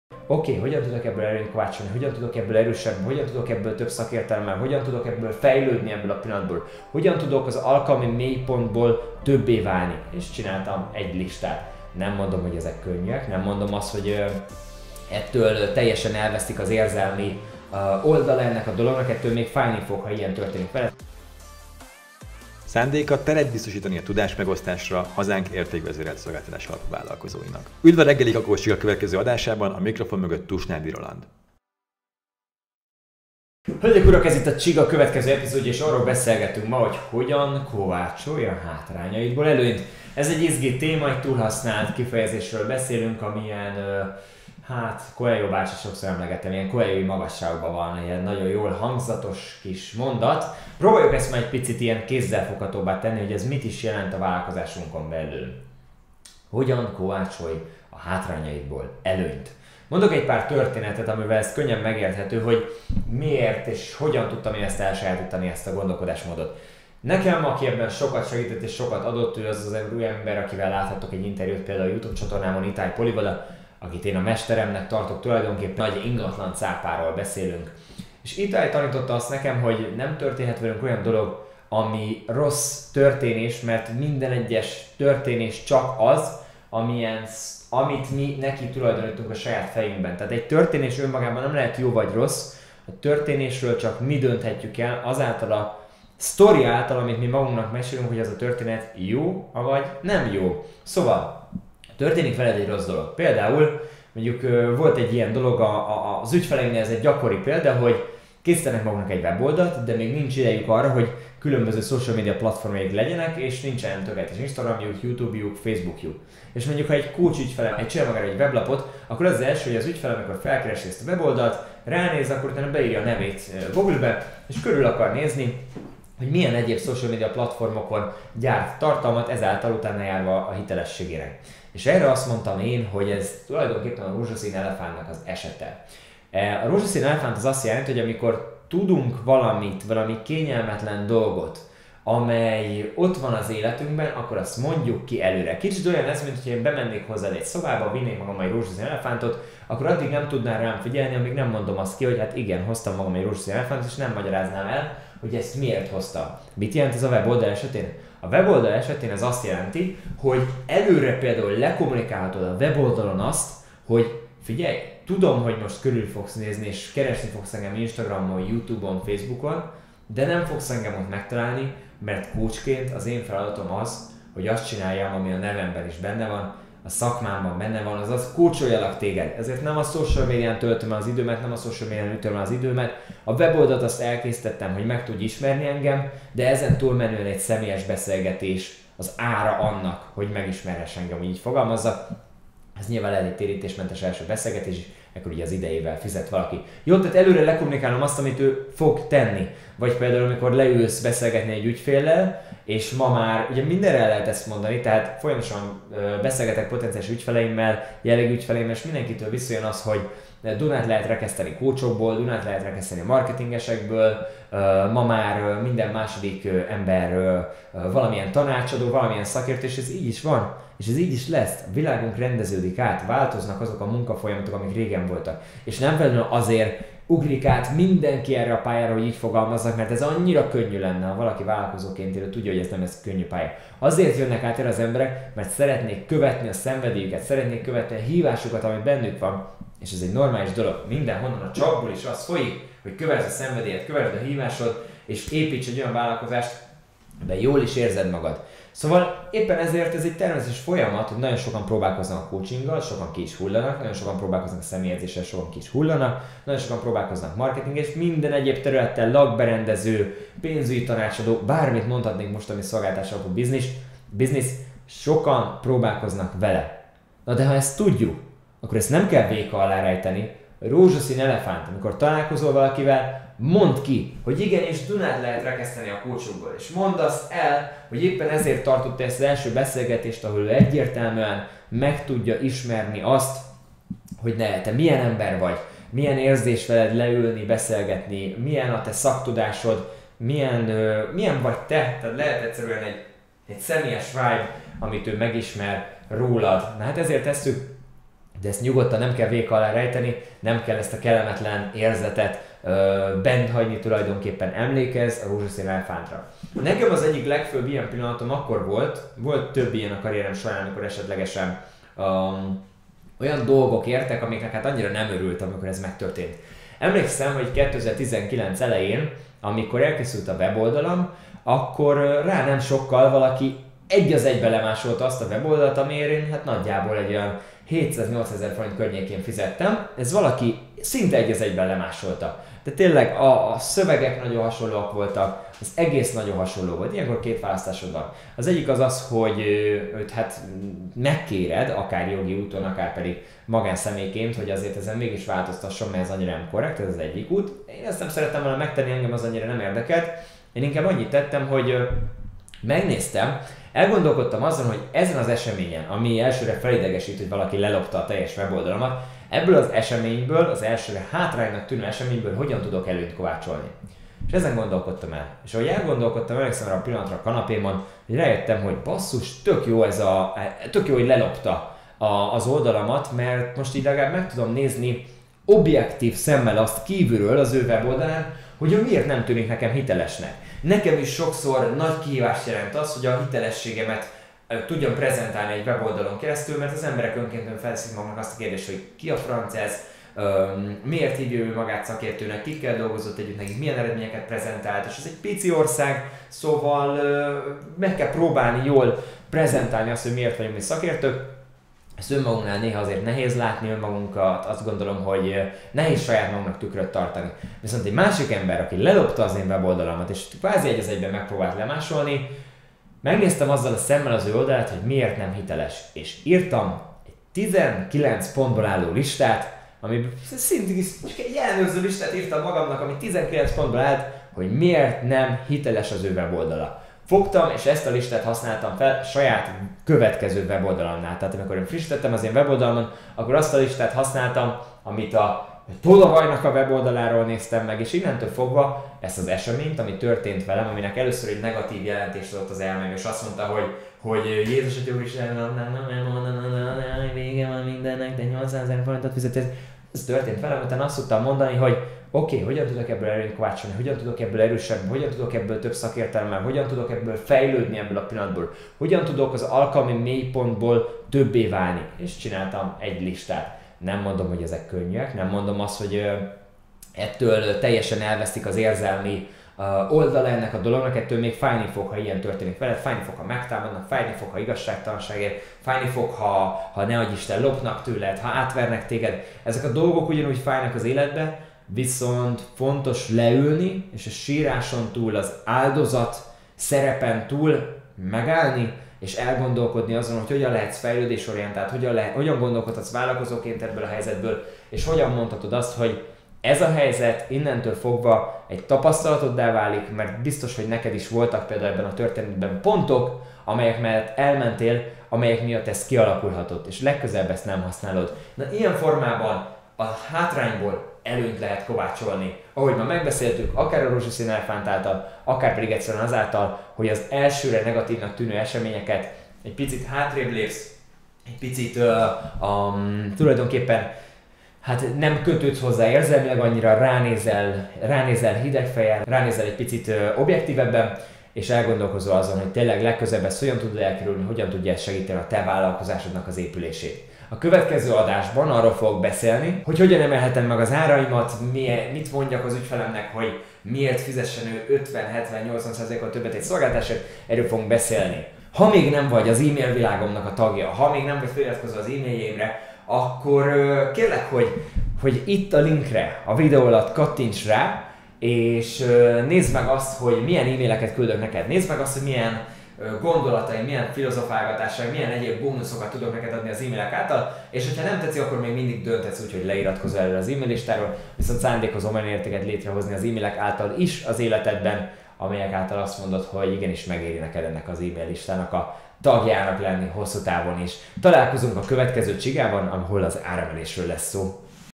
Minden bokorban marketinges/edző/coach terem? 👀| Reggeli Kakaós Csiga 8. rész
A "Reggeli Kakaós Csiga" névre keresztelt napi podcastem célja, hogy tudásommal és tapasztalatommal segítsem hazánk szolgáltatás alapú vállalkozóit és teret biztosítsak nekik a tudásmegosztásra.